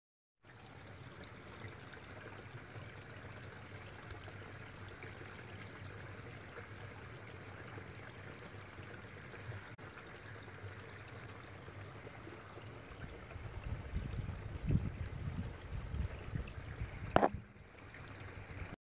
koi carp, ghost koi carp, goldfish, roach in my pond..
a video of sone larger kohaku koi carp and ghost koi carp a few goldfish and so e roach and baby cross bread koi